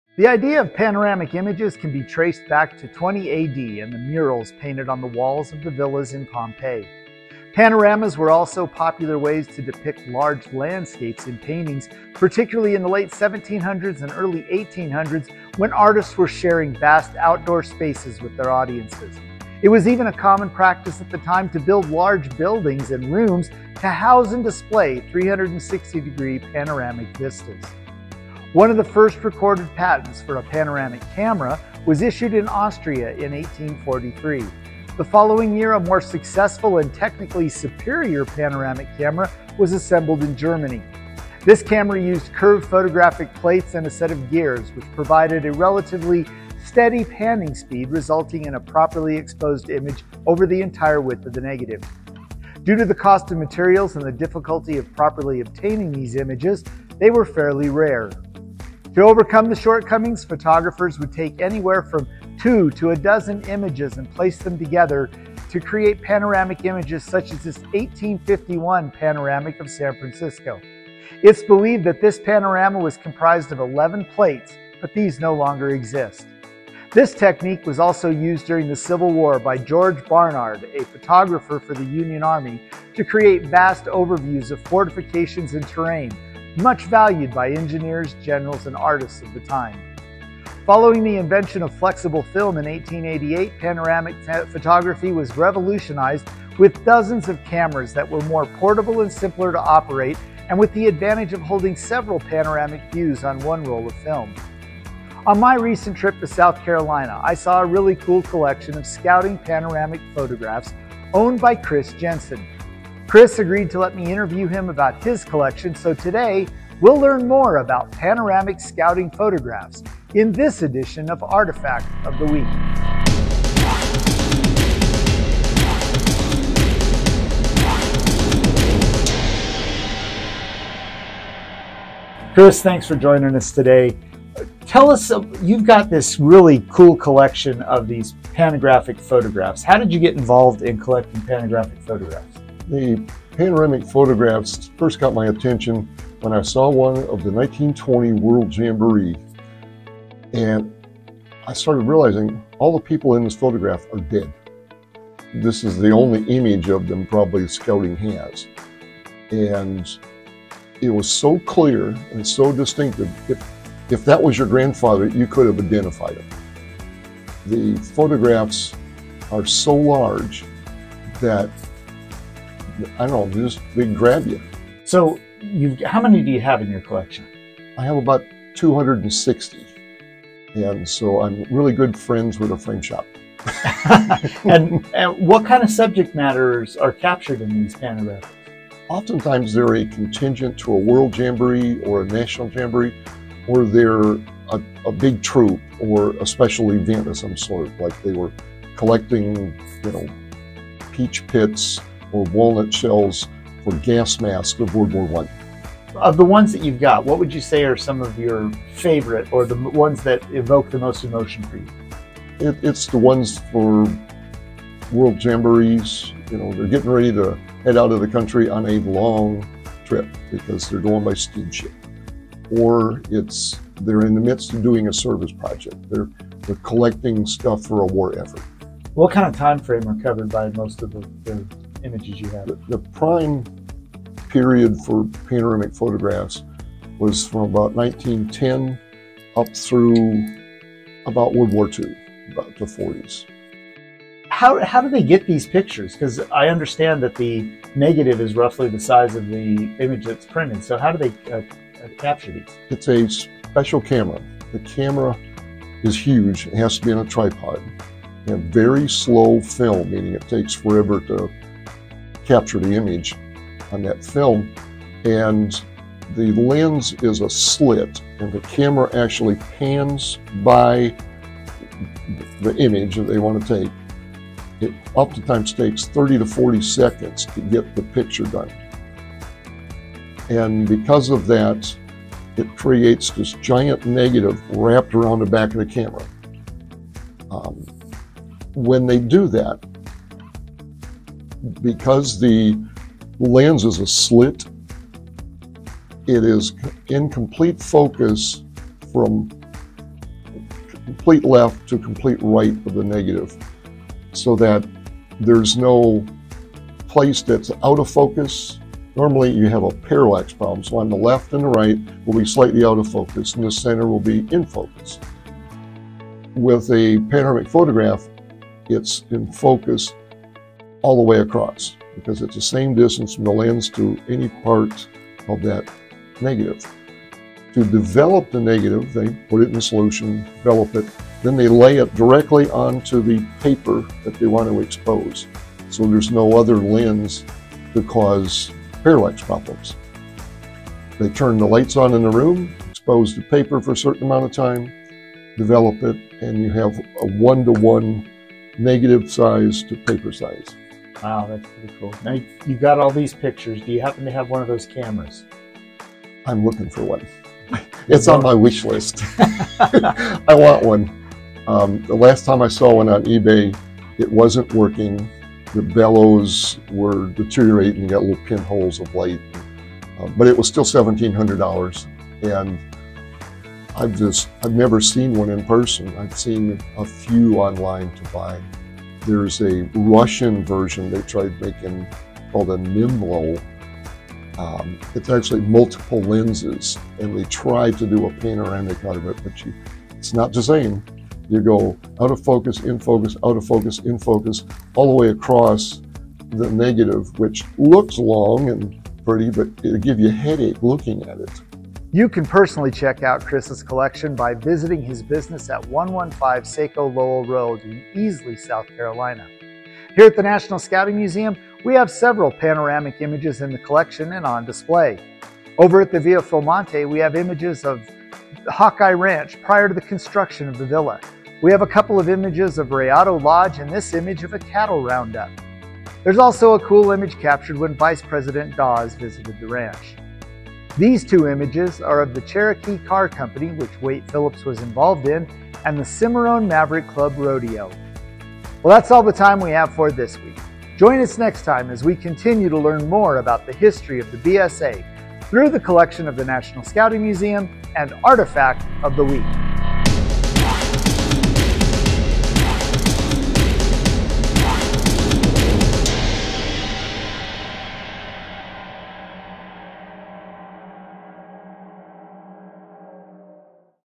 Artifact of the Week 20230324 - Panoramic Scouting Photos
Almost every modern smartphone can create amazing panoramic photos, and prior to that, you could use image editing software to “stitch” together multiple images to create a panoramic image if you wanted to. Also, there is a subset of collages called “joiners” or “panographs” that use physical images which are overlapped to create a new panoramic image, with the seams between the photos becoming a part of the newly composited image. But from the early 1900s to the 1950s, panoramic photos and the cameras that took these images were fairly common, and one Scouter collects and displays these Scouting images at his business in Easley, South Carolina. Meet Chris Jensen and his 260 panoramic Scouting Photos.

You can check these out in person at his business, Smart Storage, 115 Saco Lowell Rd., Easley, South Carolina.